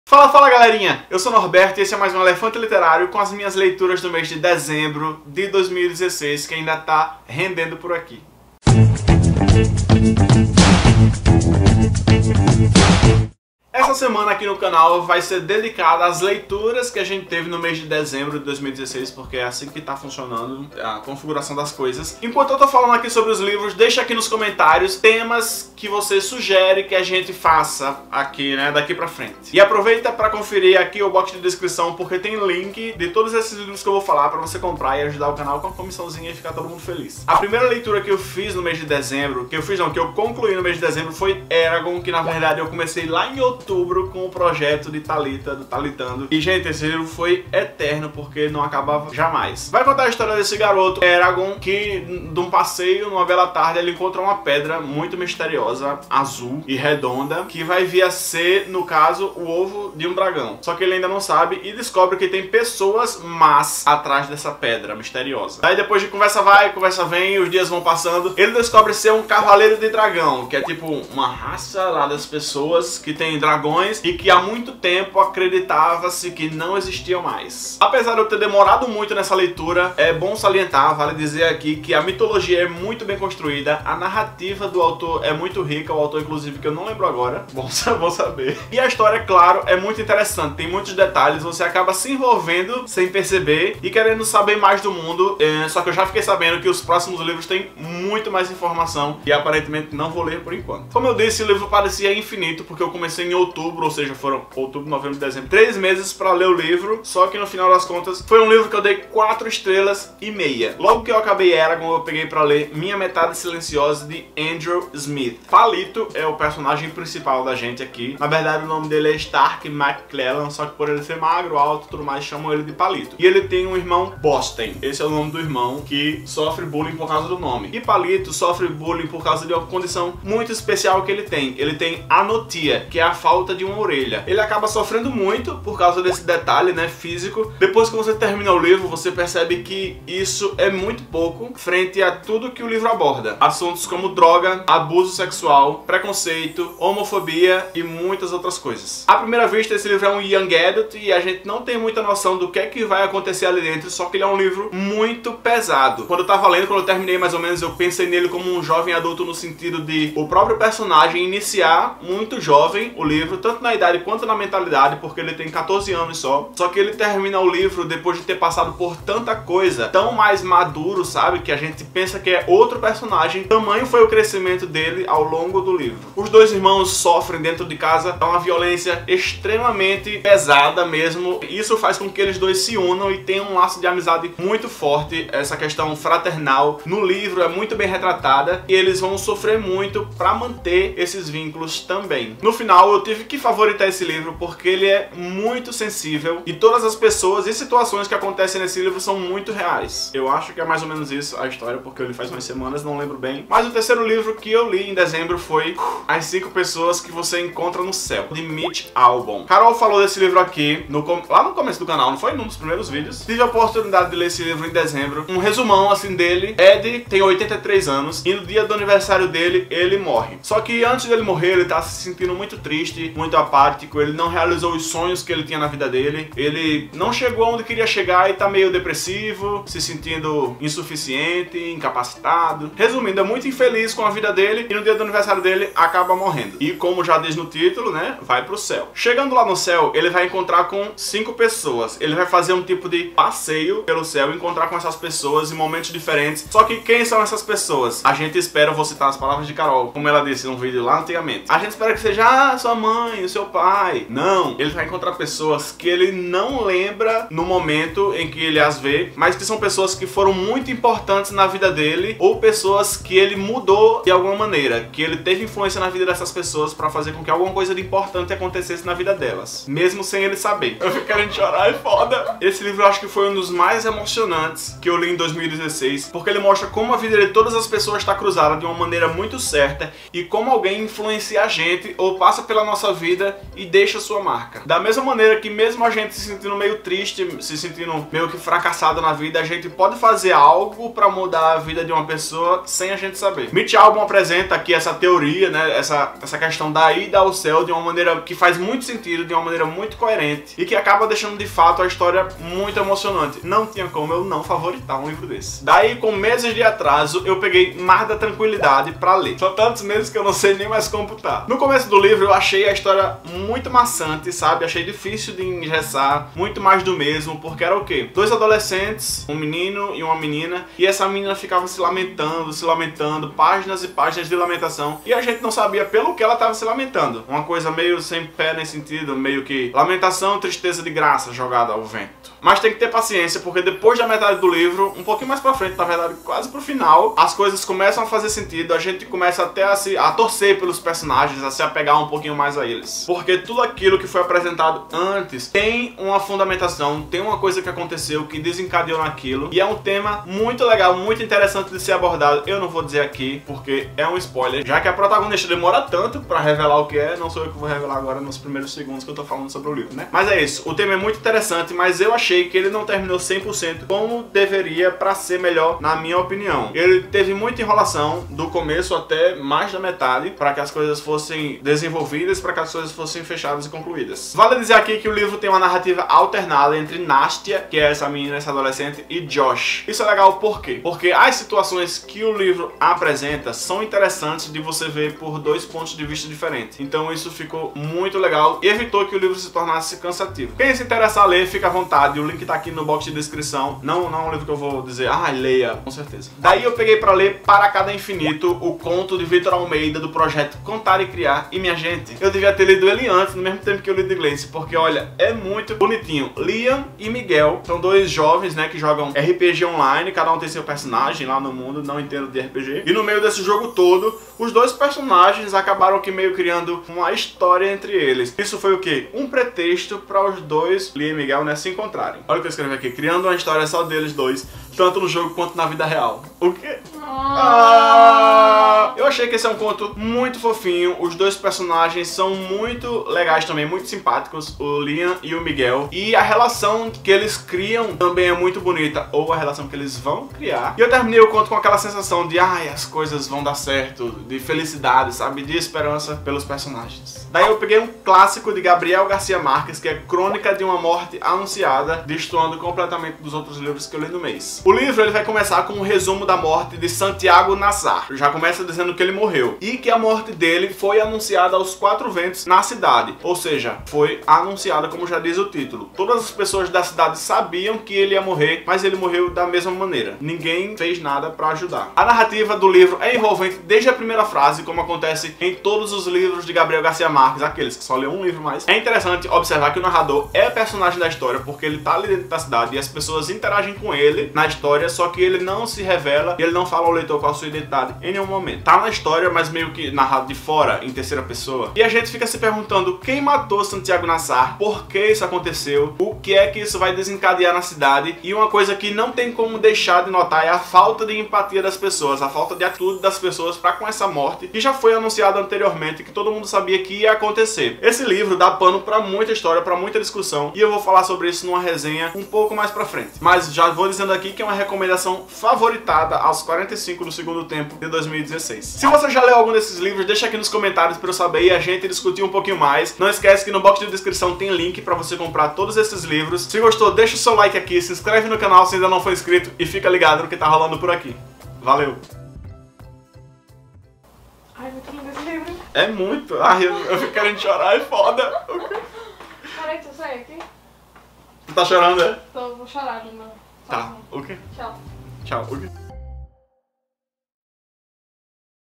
ÚLTIMAS LEITURAS DE 2016 | Norbe | Elefante Literário
Fala, fala, galerinha! Fiz leituras maravilhosas em dezembro de 2016 e achei que era uma boa compartilhar com todo mundo. Aproveita aí, que o mês foi cheio de coisa boa! :)

Compre qualquer livro desejado na Amazon através do nosso s2

➡ Compre os livros citados:

Eragon, de Christopher Paolini
As cinco pessoas que você encontra no céu, de Mitch Albom
Minha metade silenciosa, de Andrew Smith
Crônica de uma morte anunciada, de Gabriel García Marquez
Mar da tranquilidade, de Katja Milay

🖊 Se quiser compartilhar amor com o Elefante:

Elefante Literário
Caixa Postal 1503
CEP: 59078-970
Natal/RN